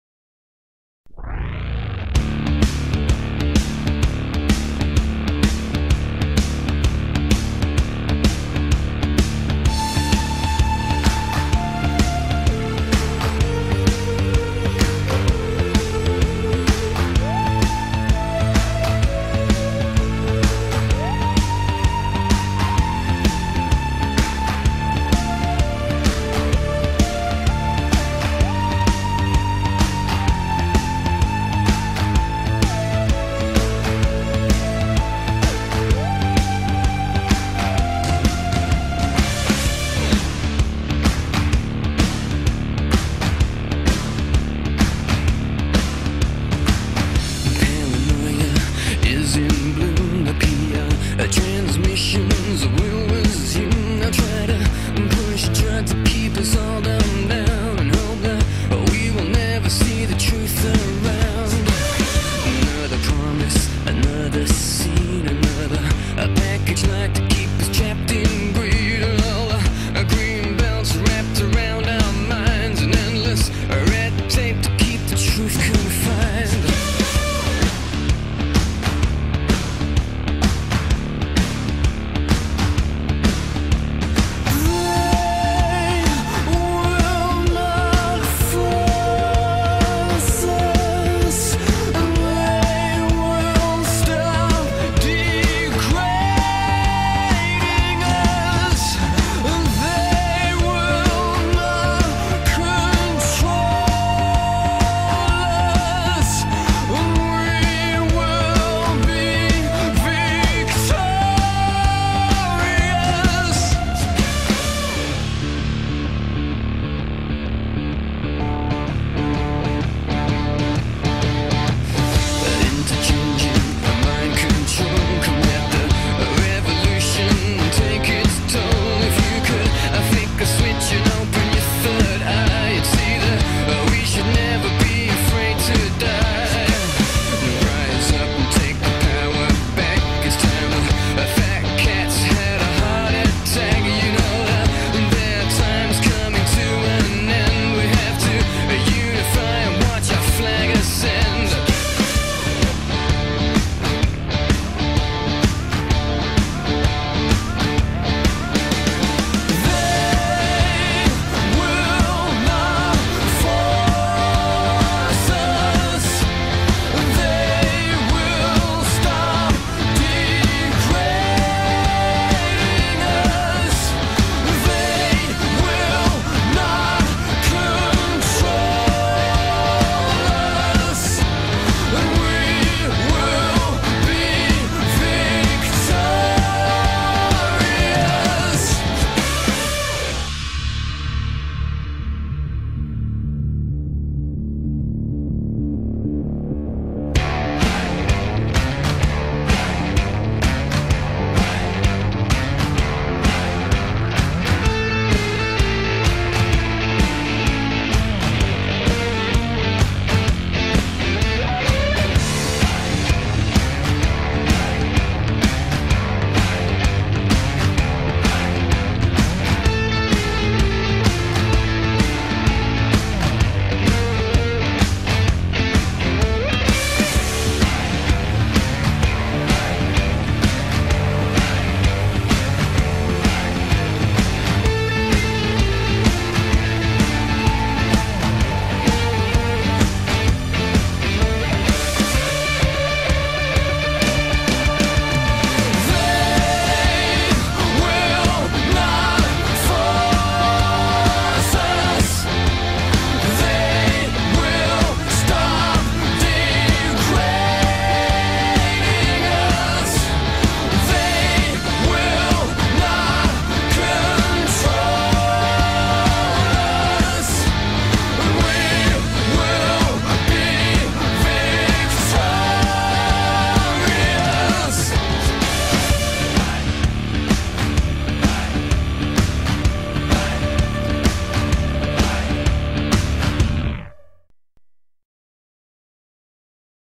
Uprising
An AV of some of the shots I took at the anti-austerity marches in London, Birmingham, & Manchester between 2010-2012.
I experimented with a Hyper-Warp technique, whereby adjacent shots from a sequence of shots have been blended together to create a 3rd dimension to the images.

I also made the 'Cinderloo Remembered' AV using some of the shots from these marches, which tells the story behind the Telford and Wrekin Unite union banner which is featured in several of the shots above